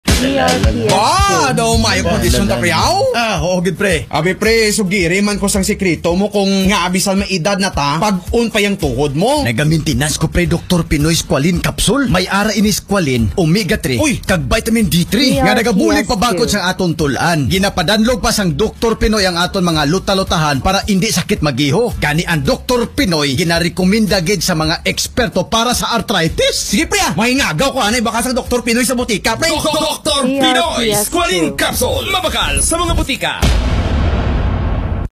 Doktor Pinoy Squalene Radio Ad 2023 35s (in Hiligaynon/Version 3)
Ito po ang ikatlong bersyon ng 2023 Radio Ad ng Doktor Pinoy Squalene Capsule, isang herbal food supplement mula sa Doktor Pinoy Pharmahealth, Inc., isang Pharamaceutical Company sa Bacolod City.

Mabakal ang Doktor Pinoy Squalene Capsule sa inyong mga botika og farmacia sa inyong mga lugar.

DRPS Two 3530: Doktor Pinoy Squalene Radio Ad 2023 35s (in Hiligaynon/Version 3)